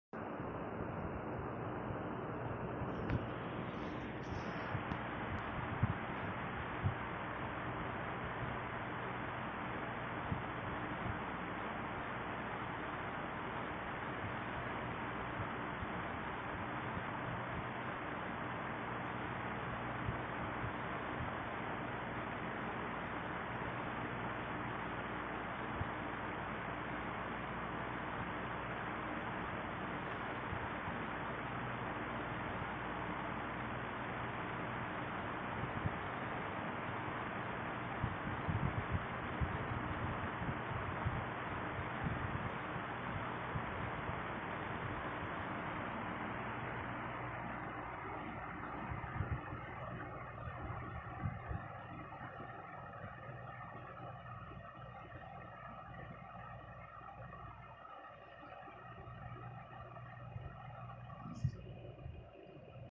Online speed drawing
This is a drawing I did a few weeks ago I decided to post it today enjoy the video